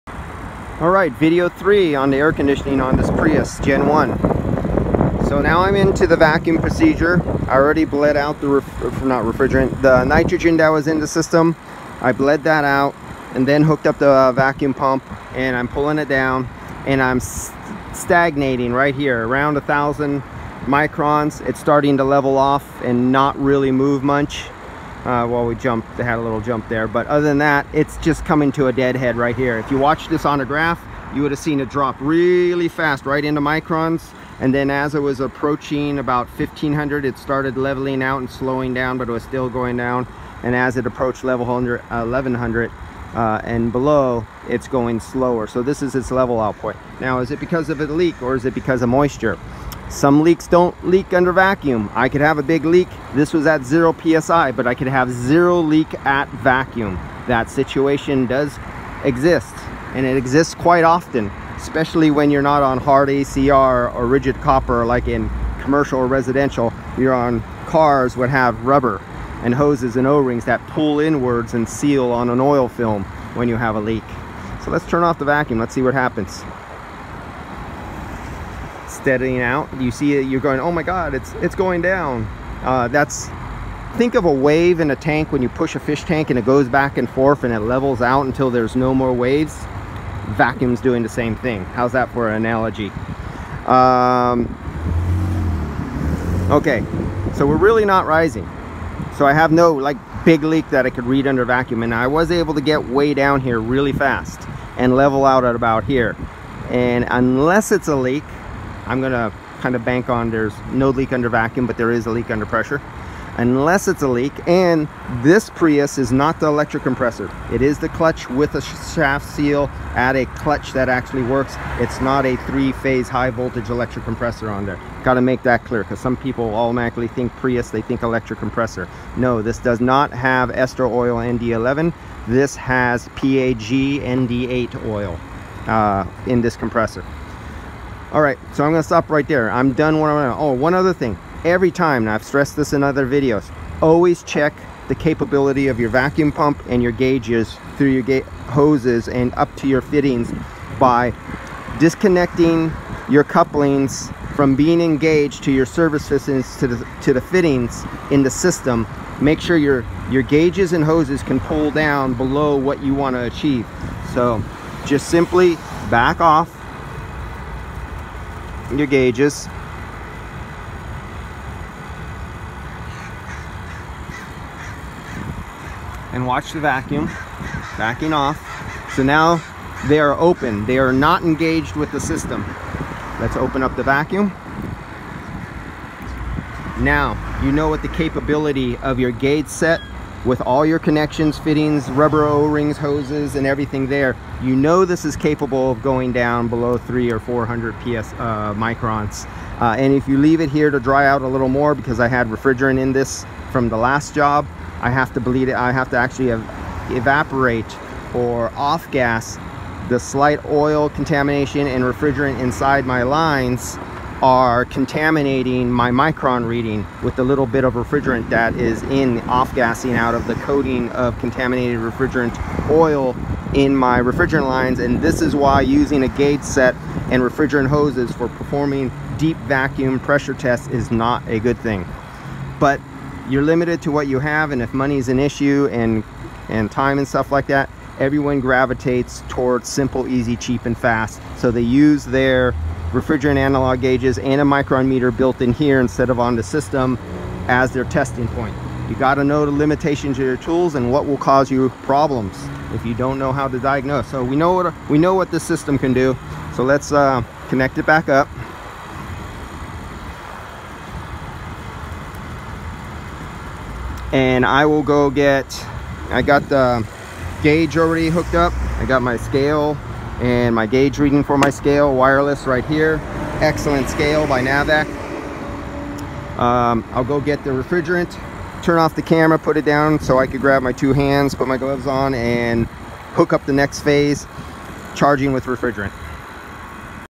Vacuum leak testing Prius GEN one looking for the easy large leak can spot moisture contamination to
Performing vacuum prior to recharging with refrigerant